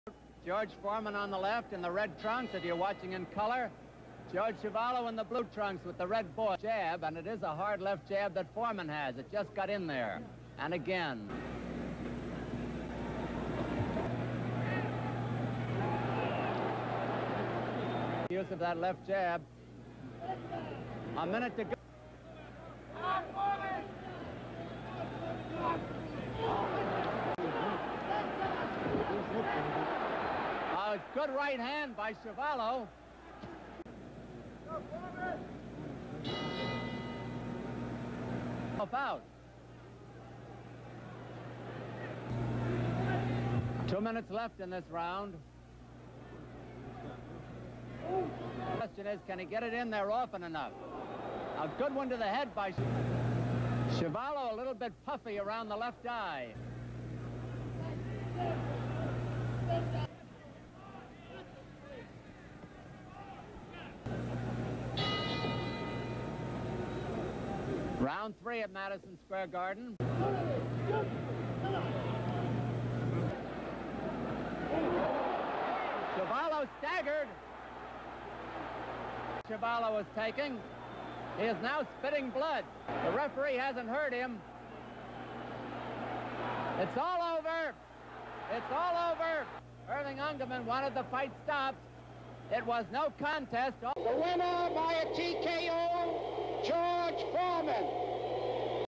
George Foreman Vs George Chuvalo Highlights (A Big Victory)
"Big" George Foreman Vs George Chuvalo (Campeón de Canadá). Pelea Por Posicion de Rankings de Peso Pesado.